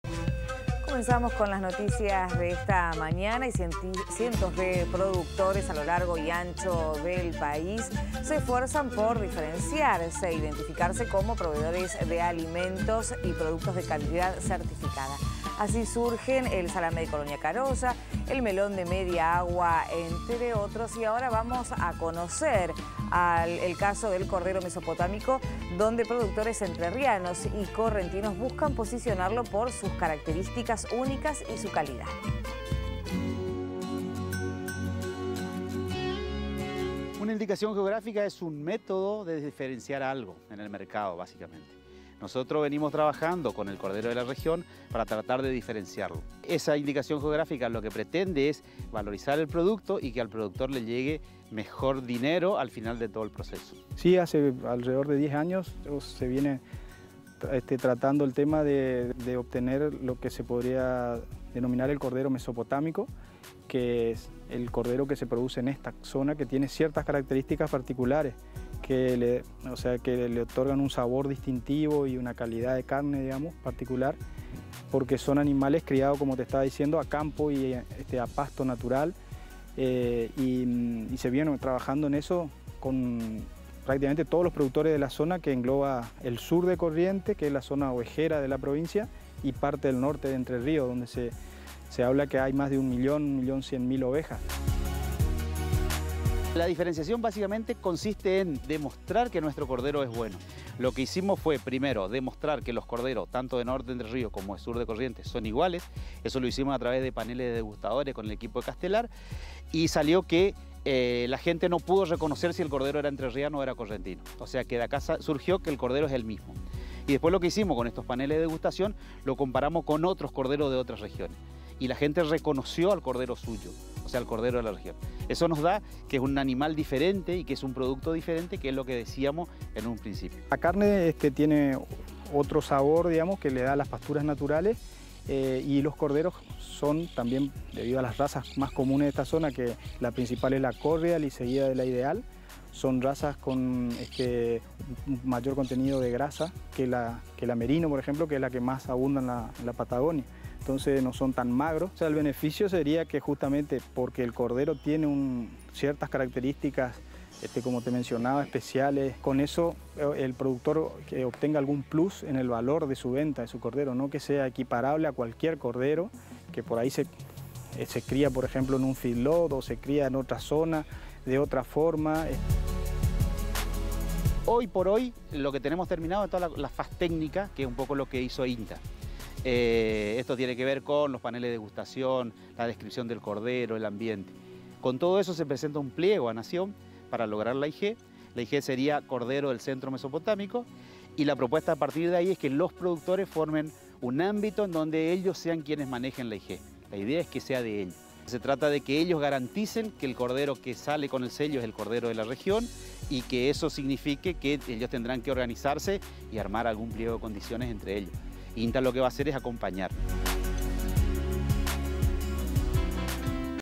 El cordero que quiere diferenciarse
Cientos de productores, a lo largo y ancho del país, se esfuerzan por diferenciarse e identificarse como proveedores de alimentos y productos de calidad certificada. Así surgen el salame de Colonia Caroya, el melón de Mediagua y el Chivito criollo del norte neuquino. Vamos a conocer el caso del cordero Mesopotámico donde productores entrerrianos y correntinos buscan posicionarlo por sus características únicas y su calidad.